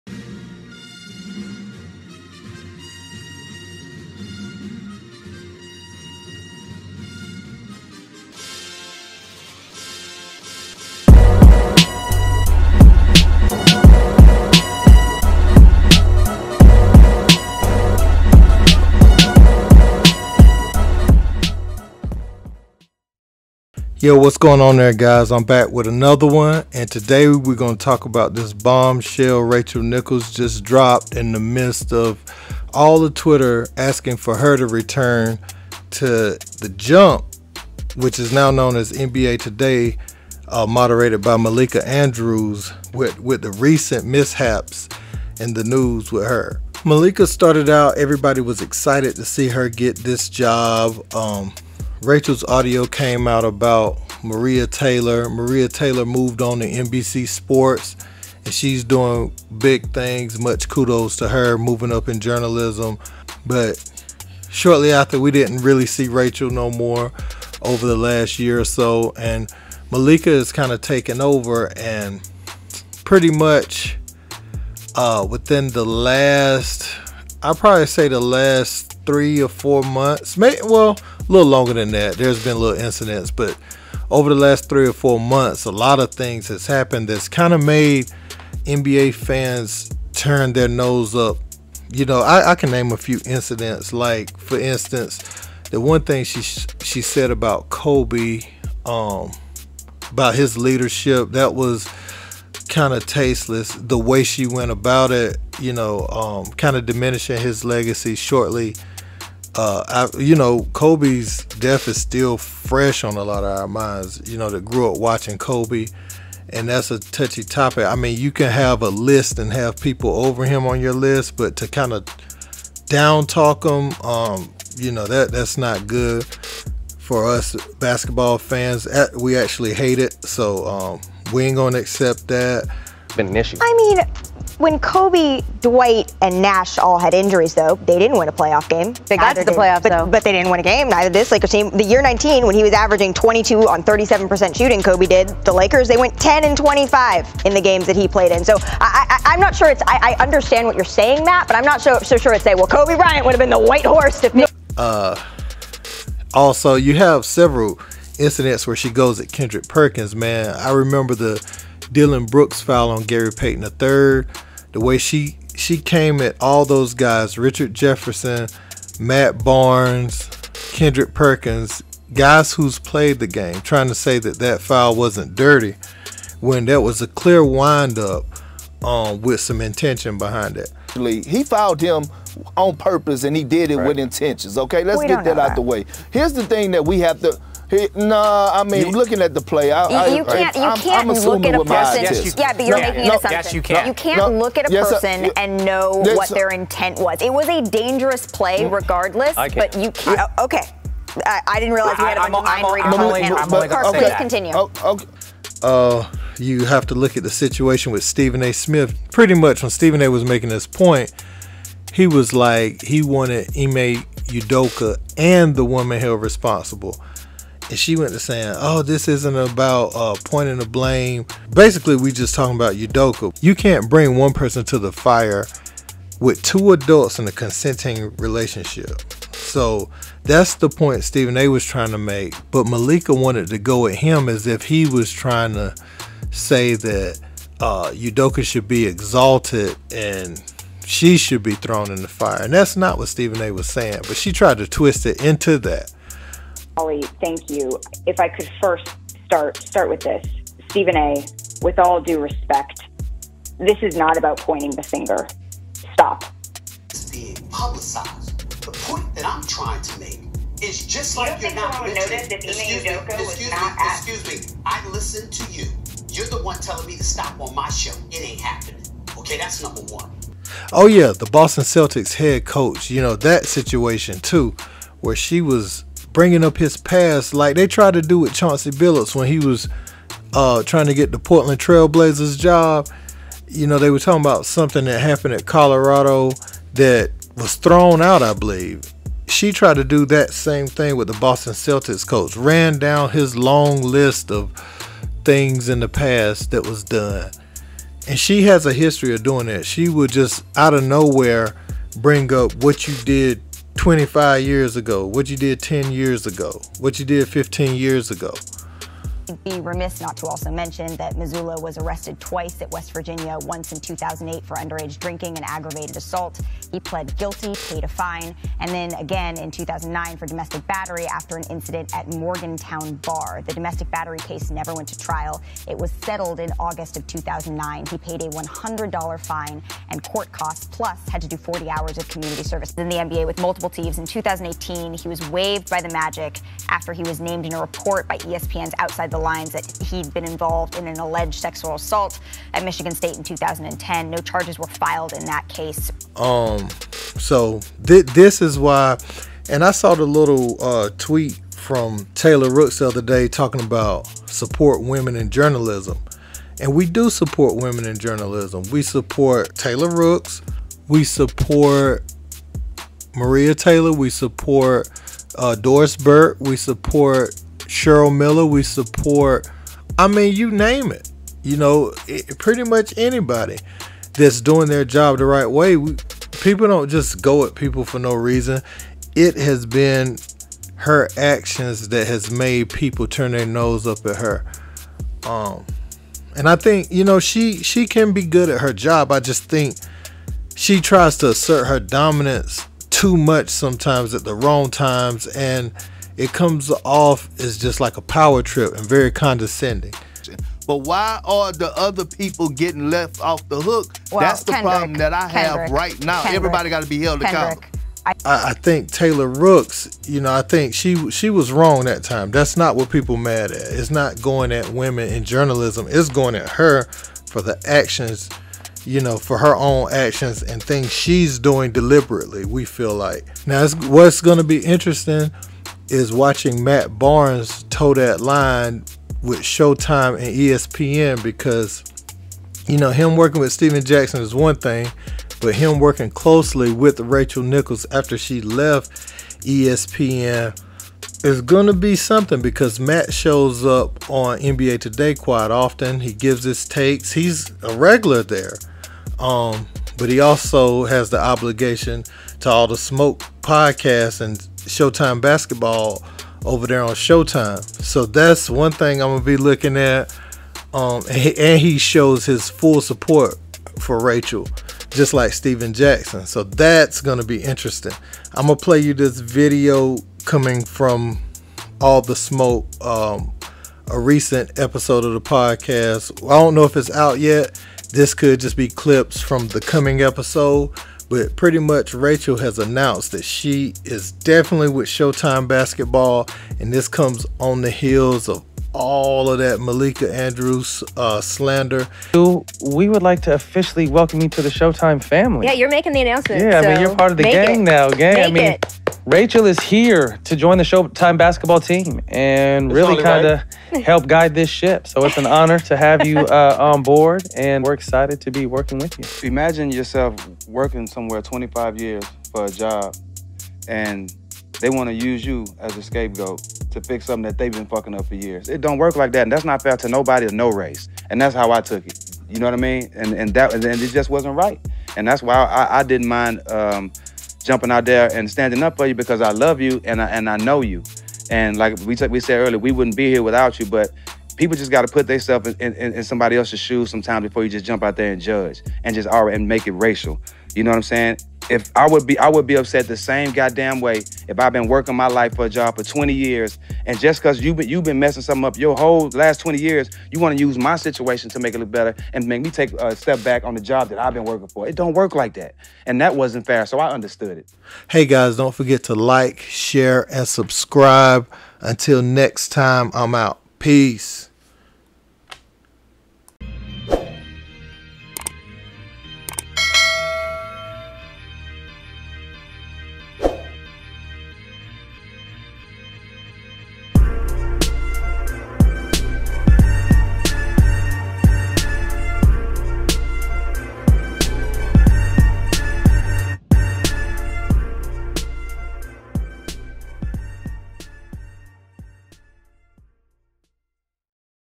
Rachel Nichols DROPS BOMB After Malika Andrews GETS CHECKED For Stephen A Smith & Ime Udoka!
Rachel Nichols delivered a big announcement about her future in sports journalism on today as many people were calling for her return. Malika Andrews has been pretty controversial to viewers with how she speaks to the former NBA players that are now journalist and well respected members of the media such as Stephen A Smith. Nichols will now partner with Showtime Basketball and work closely with Matt Barnes and Stephen Jackson.